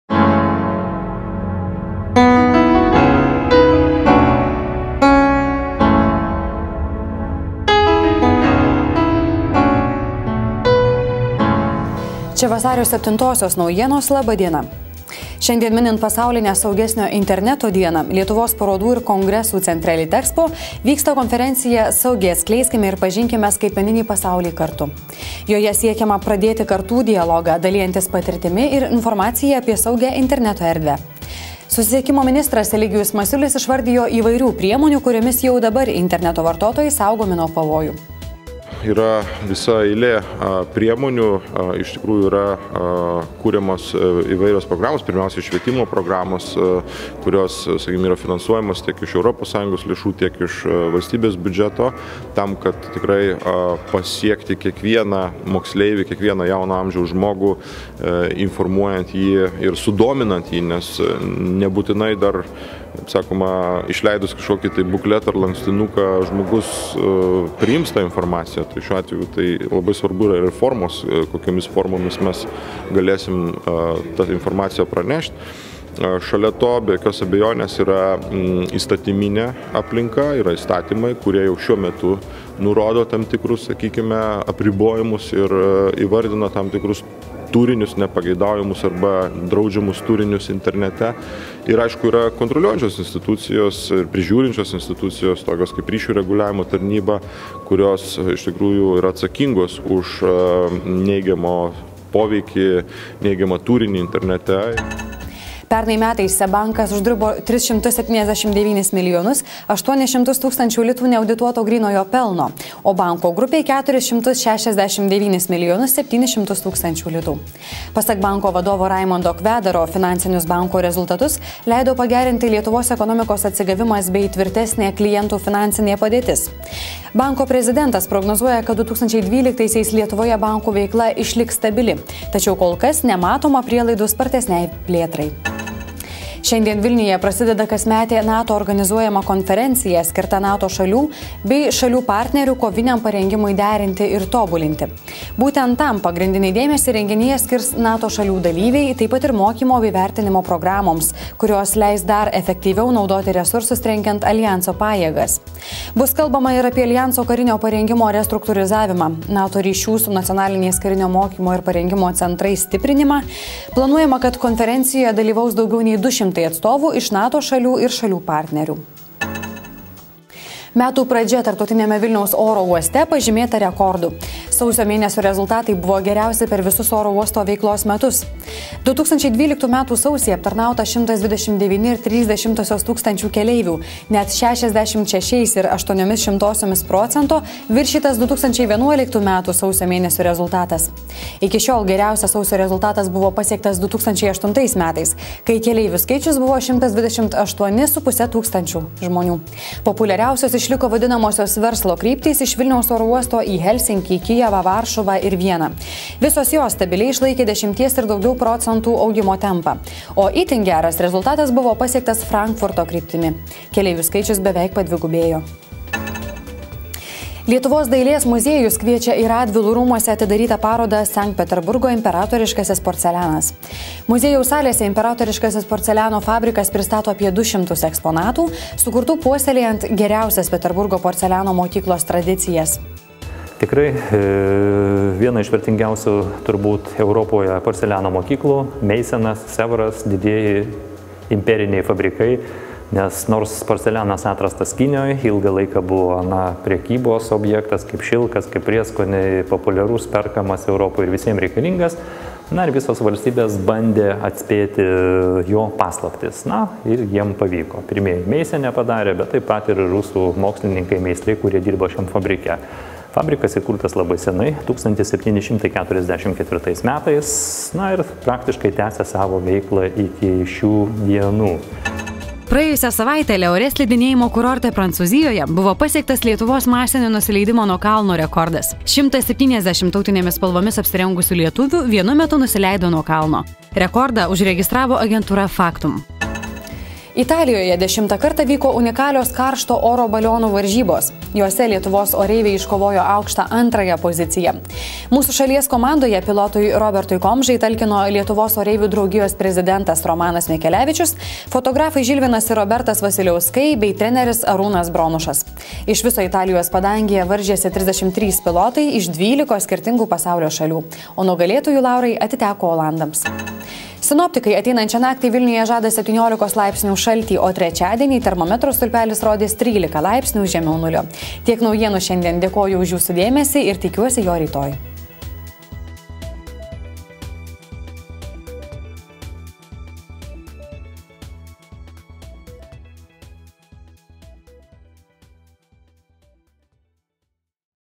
Dienos naujienos
Minint pasaulinę saugesnio interneto dieną vyksta konferencija „Saugiai atskleiskime ir pažinkime skaitmeninį pasaulį kartu". 2011-aisiais SEB bankas uždirbo 379,8 mln. Lt pelno. Vilniuje prasideda kasmetinė NATO konferencija. Tarptautiniame Vilniaus oro uoste sausio mėnesio rezultatai buvo geriausi per visus oro uosto veiklos metus.  Lietuvos dailės muziejus kviečia į Radvilų rūmuose atidarytą parodą „Sankt Peterburgo imperatoriškasis porcelianas".